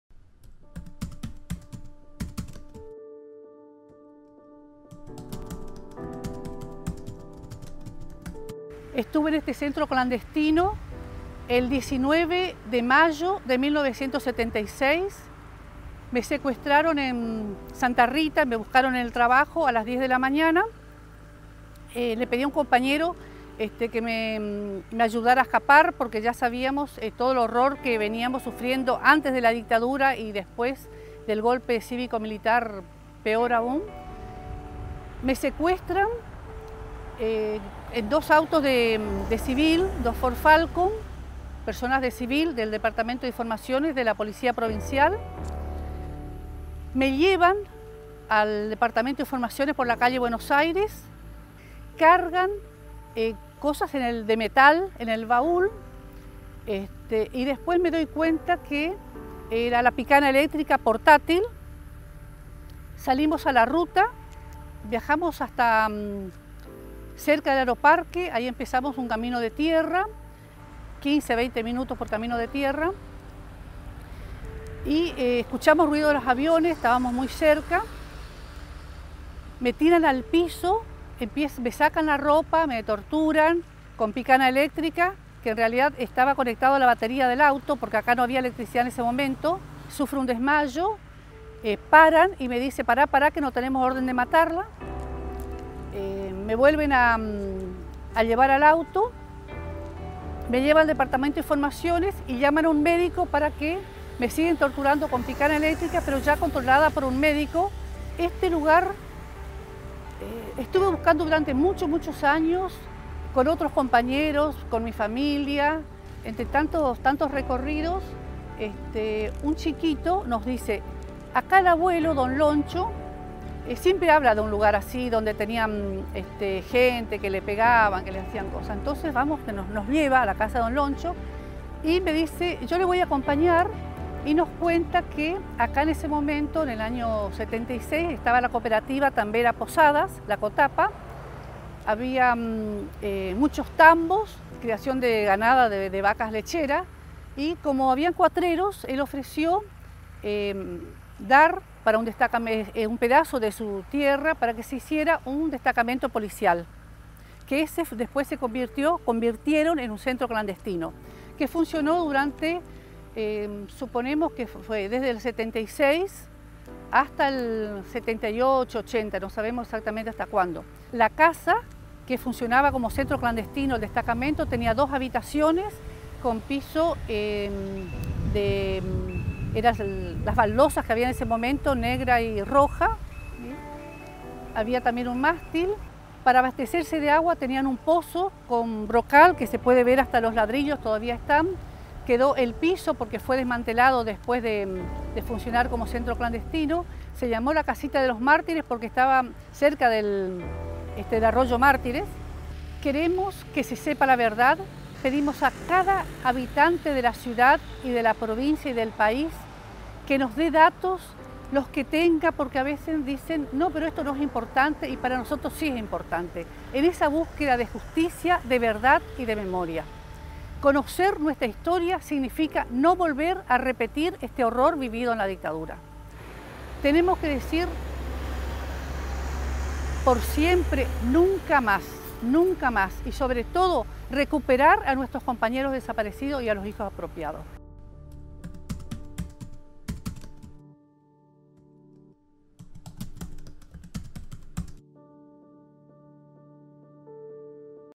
Casita de Mártires
Ex centro clandestino de detención y tortura: Casita de Mártires. ID 625. Dirección: Av. Ulises López y Chacabuco.